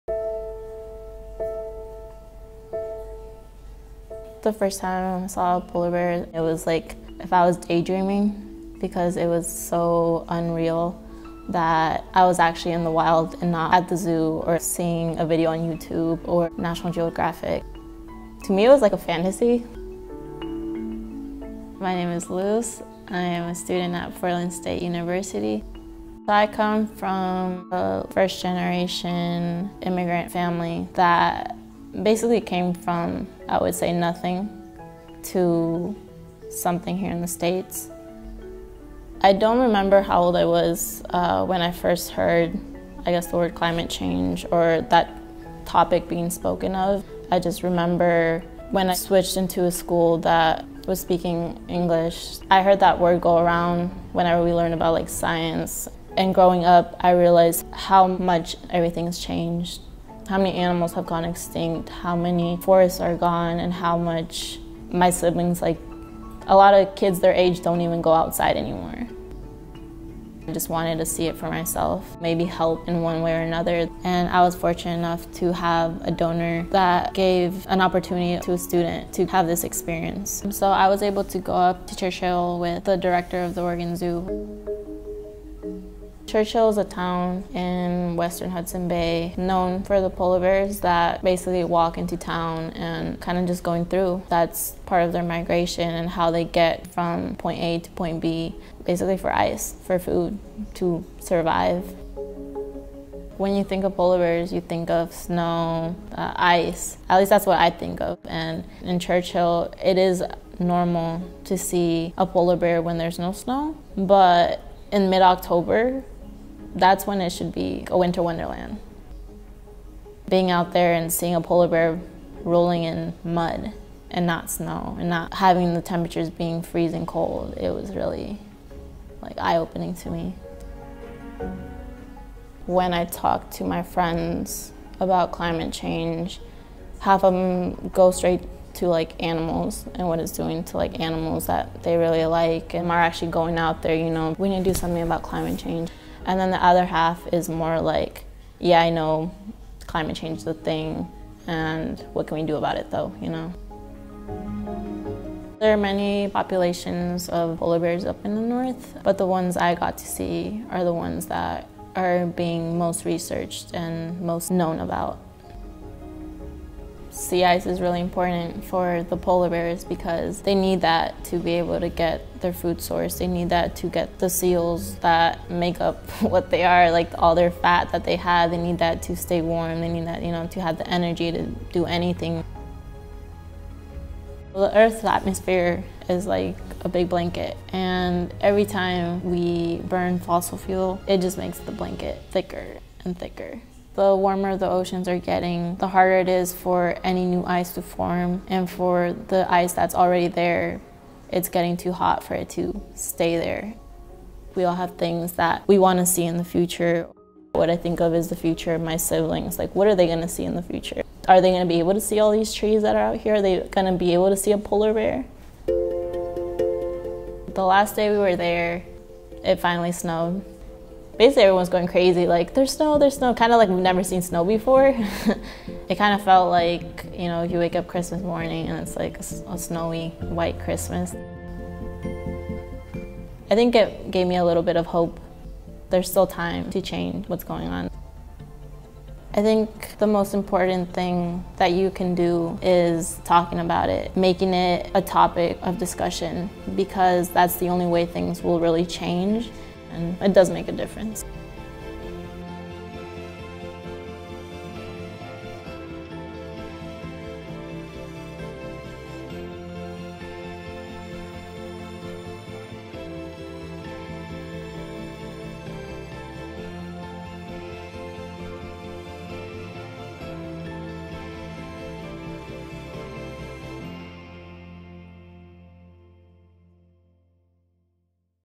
Seeing a Polar Bear for the First Time
In 2019, the Oregon Zoo and Oregon Zoo Foundation traveled with 16 zoo supporters to Churchill, Manitoba: the polar bear capital of the world.
Portland State University undergraduate Luz Mejia-Flores joined the group as a visual storyteller. During the trip, Luz met with zoo staff and conservation scientists and documented her experience through this video.

The trip was led by Oregon Zoo Director Dr. Don Moore, who shared scientific knowledge and expertise gathered over many decades working with polar bears and endangered species. This project was generously funded by Oregon Zoo Foundation donors.